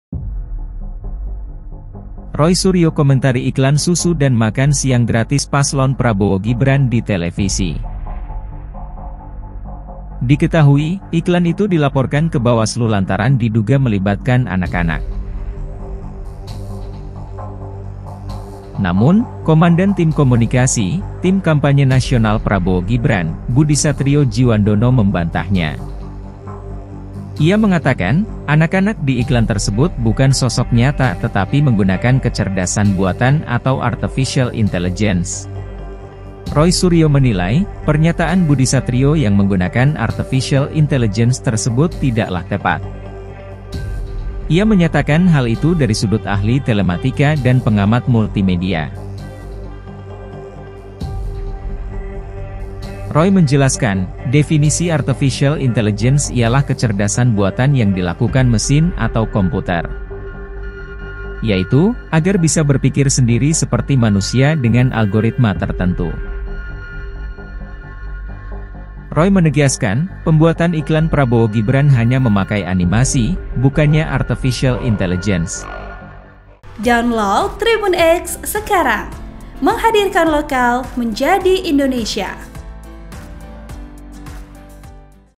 Roy Suryo Semprot soal Iklan Prabowo-Gibran di TV yang Disebut Pakai AI: Jangan Sok Paham
TRIBUN-VIDEO.COM - Pakar telematika Roy Suryo, turut mengomentari iklan susu dan makan siang gratis pasangan calon presiden (capres)-calon wakil presiden (cawapres) Prabowo Subianto-Gibran Rakabuming Raka di TV nasional yang menuai kontra.

Iklan politik itu dilaporkan kelompok masyarakat Radar Demokrasi Indonesia (RDI) ke Badan Pengawas Pemilu (Bawaslu) karena diduga melibatkan anak-anak di dalamnya.

Namun, Komandan Tim Komunikasi Tim Kampanye Nasional (TKN) Prabowo-Gibran, Budisatrio Djiwandono, mengatakan anak-anak dalam iklan tersebut bukanlah sosok nyata melainkan dibuat menggunakan kecerdasan buatan atau artificial intelligence (AI).

"Tidak ada anak-anak yang dilibatkan dalam pembuatan video iklan tersebut. Ini murni kreasi Artificial Intelligence atau AI dari teks menjadi gambar yang di-generate melalui AI,” ujar Budi kepada wartawan, Rabu (22/11/2023).

Roy Suryo menilai pernyataan juru bicara TKN Prabowo-Gibran yang menggunakan istilah AI tidaklah tepat.

"Menggunakan istilah AI adalah salah kaprah dan justru menggelikan (untuk tidak mengatakannya memalukan)."

"Bagaimana tidak? Harusnya yang dimaksud teknologi yang digunakan dalam pembuatan iklan tersebut adalah animasi, bukan AI," ungkap Roy Suryo kepada Tribunnews, Kamis (23/11/2023).

Roy Suryo menyatakan pandangannya murni dari sudut ahli telematika dan pengamat multimedia, karena tak lagi berkecimpung di politik praktis.

Roy Suryo menjelaskan, definisi AI adalah sistem kecerdasan buatan yang dilakukan oleh mesin atau komputer untuk bisa berpikir sendiri seperti manusia dengan algoritma tertentu.

"Misalnya adalah komputer DeepBlue yang pernah ditandingkan melawan pecatur dunia asal Rusia beberapa waktu yang bisa 'menggunakan otak buatan' dan 'berpikir layaknya manusia'. Secara teknis juga ada dua jenis AI, strong dan weak AI, sesuai tingkat kerumitannya," urainya.

Mantan politisi Demokrat itu menegaskan pembuatan iklan Prabowo-Gibran hanya menggunakan animasi, bukan AI.

"Karena iklan tersebut jelas-jelas masih diprogram, digambar atau bahkan dipotret dan discan oleh Manusia. Artinya karakter-karakter orangnya bisa dengan cara digambar atau dari foto asli dibuat wireframenya kemudian dimodeling menjadi tokoh tertentu seperti pada bagian akhir tayangan yang mengambil sosok 'lucu dan menggemaskan'," ungkap Roy Suryo.

"Jadi kesimpulannya, kalau memang belum (tidak) paham istilah AI, sebaiknya jangan sok paham, mending gunakan saja istilah yang umum, misalnya 'Itu hanya menggunakan komputer, bukan anak-anak langsung' akan lebih bijak daripada salah istilah," ujar Roy Suryo.

Artikel ini telah tayang di Tribunnews.com dengan judul Anak di Iklan Susu Prabowo-Gibran Disebut AI, Roy Suryo: Salah Kaprah, Sembarangan Pakai Istilah,
Penulis: Wahyu Gilang Putranto
Editor: Nuryanti